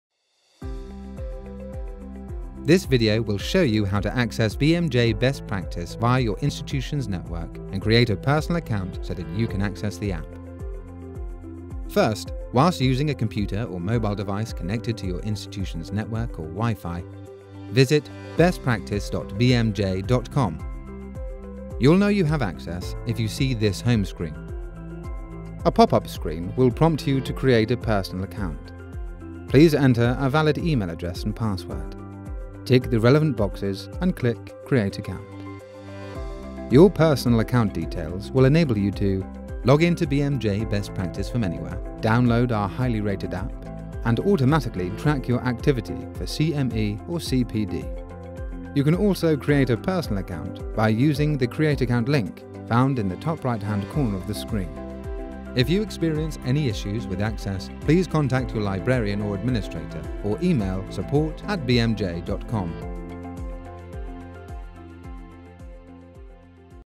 How to access BMJ Best Practice via your institution's network
A quick walkthrough of how to access BMJ Best Practice via your institution’s network (IP ranges) and register for a free personal account